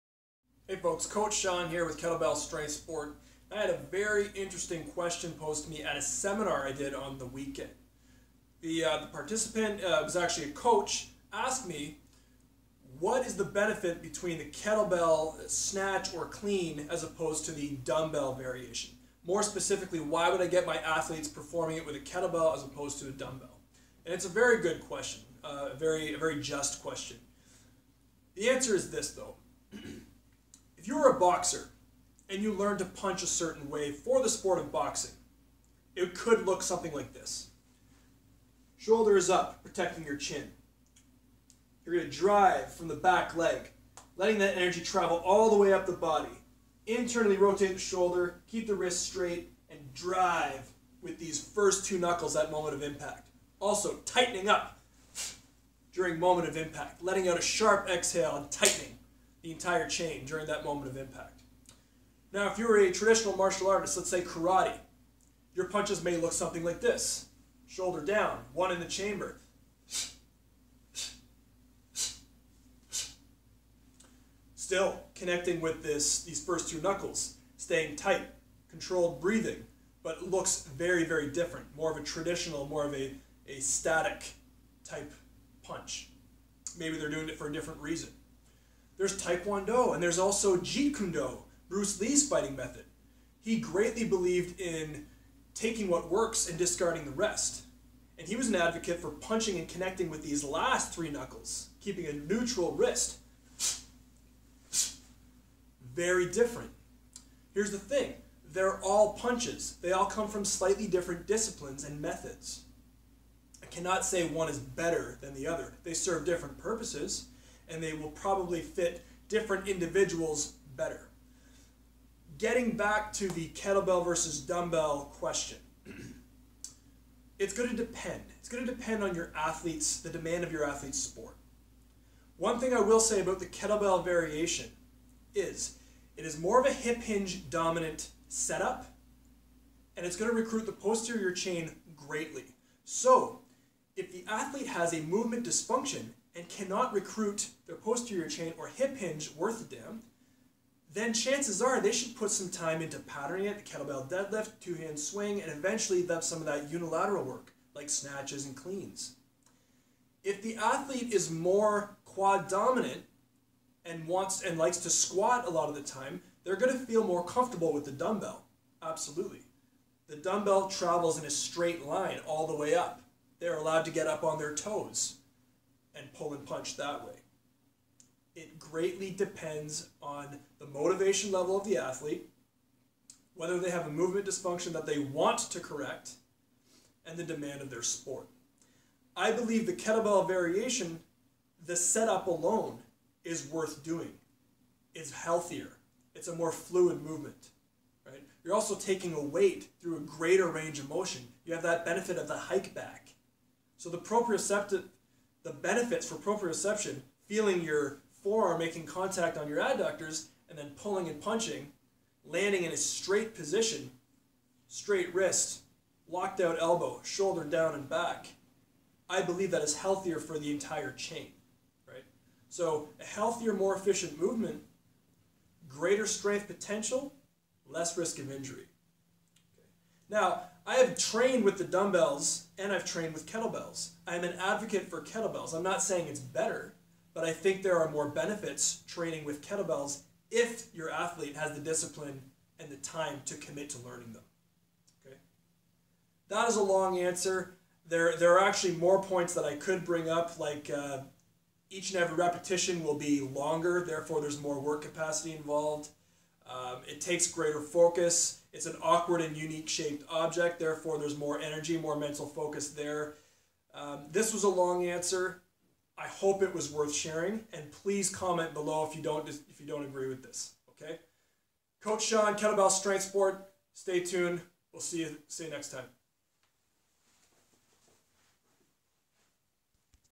Dumbbell vs Kettlebell - The Reality
Presenting some of the differences between the two tools